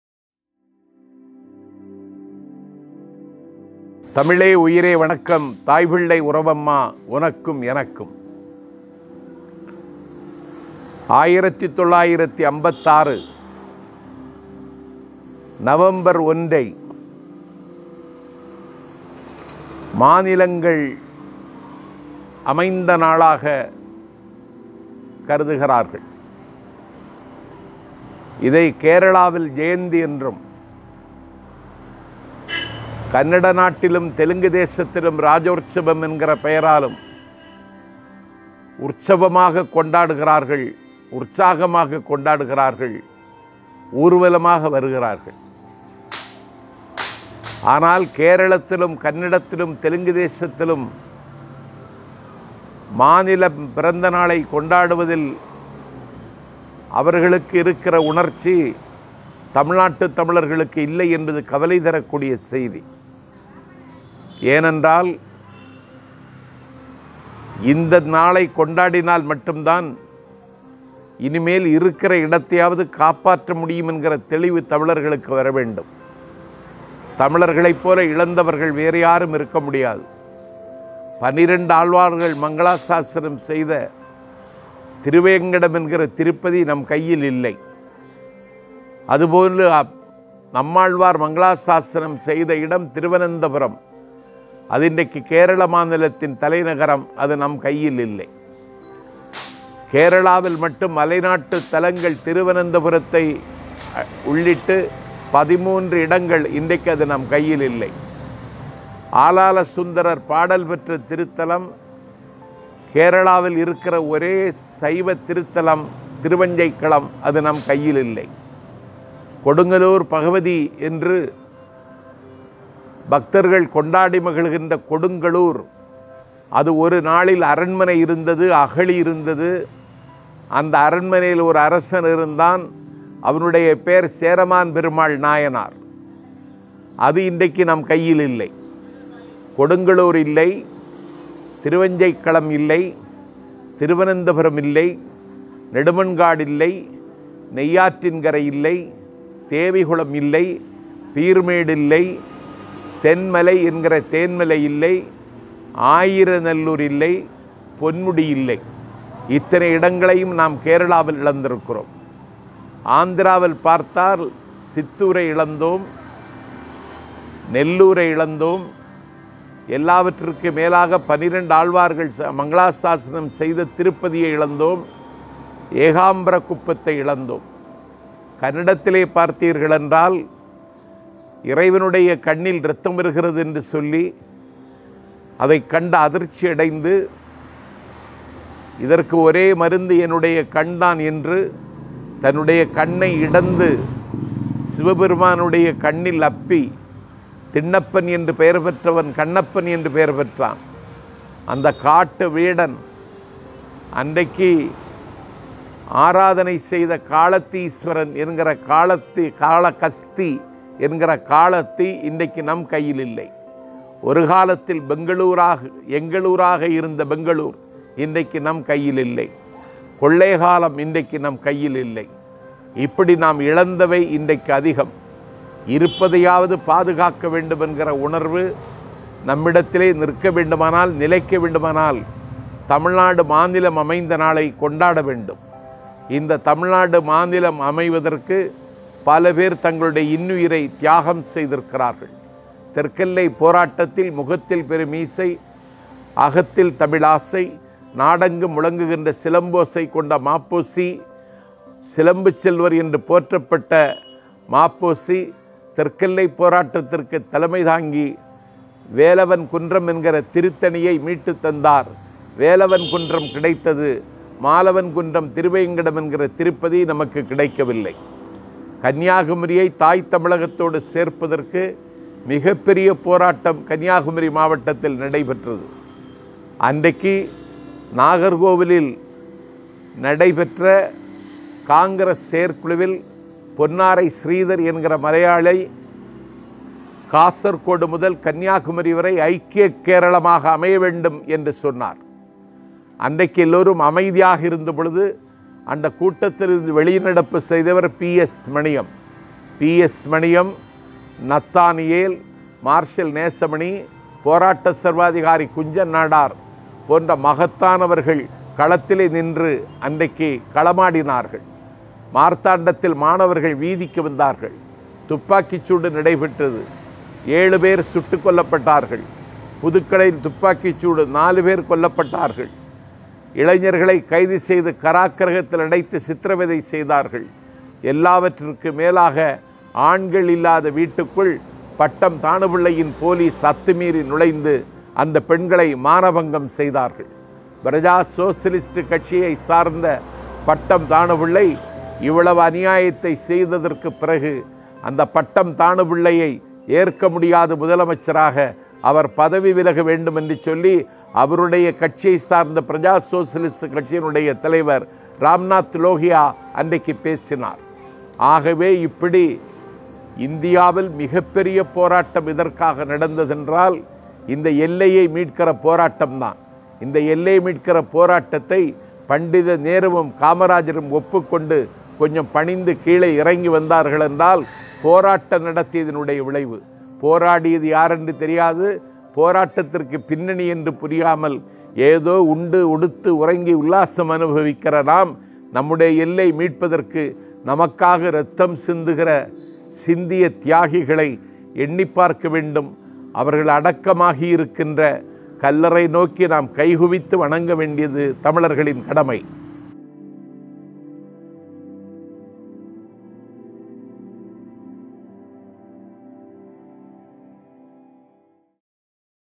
எல்லையை இழந்தோம் உள்ளதை காப்போம்! | Nanjil Sampath | Tamil
The present-day Kanyakumari district of Tamil Nadu state in India was originally a part of the Travancore-Cochin state. Between 1945 and 1956, especially after the Government of India announced plans to reorganize states along linguistic lines, the people of Tamil-majority Kanyakumari campaigned for its inclusion in the Madras State (later Tamil Nadu) instead of the Malayalam-majority Kerala state. In Tamil, the campaign is also known as Therkku Ellai Porattam ("South Frontier Struggle").

The campaign was successful: Agastheeswaram, Kalkulam, Thovalai, Vilavancode and half part of Shenkottai taluks were merged with Madras as per the States Reorganisation Act, 1956.[1] The first four were combined to form the present-day Kanyakumari district, while Shenkottai was merged with the Tirunelveli district.
Pattom Thanu Pillai was the chief minister for Thiru - Kochi legislative assembly. He engaged hard measures against the agitations of Tamils. Especially the Tamils at Devikulam - Peermedu regions went through the atrocities of Travancore Police force. Condemning the attitude of the police, T.T.N.C leaders from Nagercoil went to Munnar and participated in agitations against the prohibitive orders. The leaders were arrested and an uncalm atmosphere prevailed in South Travancore.